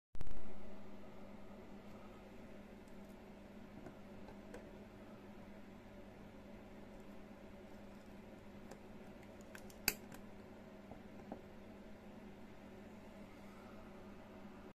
Exakta Varex IIb shutter 1080p
The cocking action for the Exakta Varex IIb shutter